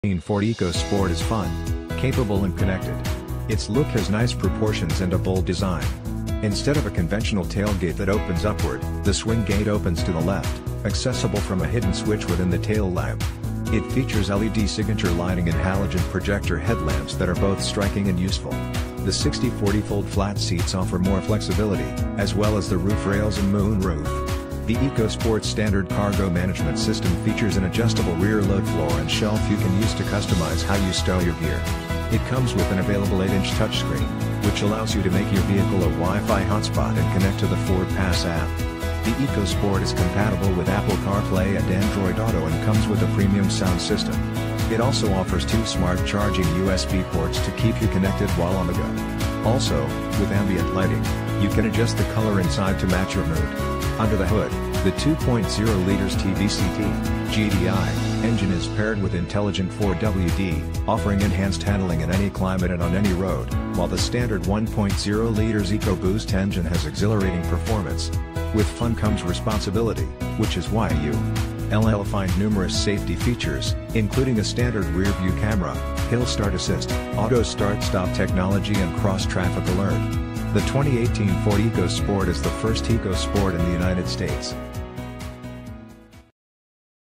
2018 Ford EcoSport 18F292
2018 FORD ECOSPORT Chapmanville, WV
Stock# 18F292

500 Ford Fairlane Corridor G, US-119

The 2018 Ford EcoSport is fun, capable and connected. Its look has nice proportions and a bold design. Instead of a conventional tailgate that opens upward, the swing gate opens to the left, accessible from a hidden switch within the taillamp. It features LED signature lighting and halogen projector headlamps that are both striking and useful. The 60/40 fold-flat seats offer more flexibility, as well as the roof rails and moonroof. The EcoSport standard cargo management system features an adjustable rear load floor and shelf you can use to customize how you stow your gear. It comes with an available 8-inch touchscreen, which allows you to make your vehicle a Wi-Fi hotspot and connect to the FordPass app. The EcoSport is compatible with Apple CarPlay and Android Auto and comes with a premium sound system. It also offers two smart charging USB ports to keep you connected while on the go. Also, with ambient lighting, you can adjust the color inside to match your mood. Under the hood, the 2.0L Ti-VCT (GDI) engine is paired with intelligent 4WD, offering enhanced handling in any climate and on any road, while the standard 1.0L EcoBoost engine has exhilarating performance. With fun comes responsibility, which is why you?ll find numerous safety features, including a standard rear-view camera, hill start assist, auto start-stop technology and cross traffic alert. The 2018 Ford EcoSport is the first EcoSport in the United States.